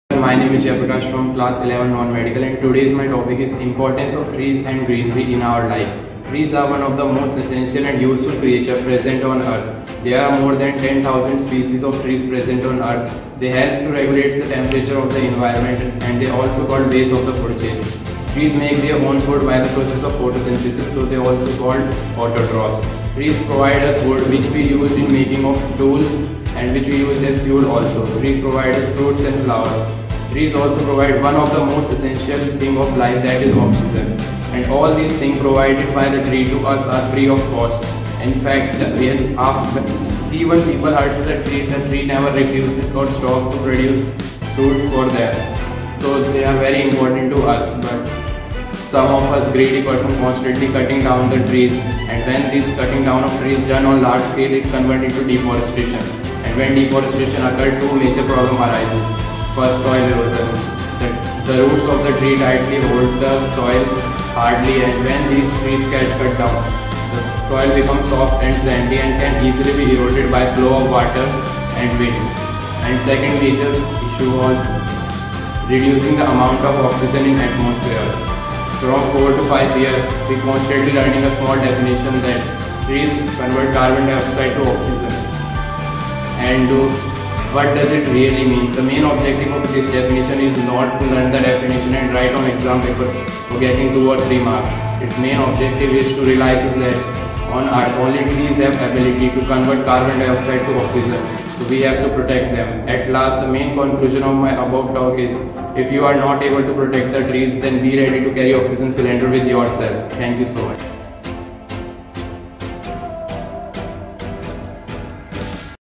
Importance of Greenery
Greenery in our living environment benefits more than just our health and well-being. It also facilitates water management and promotes biodiversity in built - up areas, and can help reduce the effect of noise pollution. Greenery also helps to raise the property value of homes and offices.

The remarkable advantage of greenery is explained by Jaiprakash Yadav 11(NM)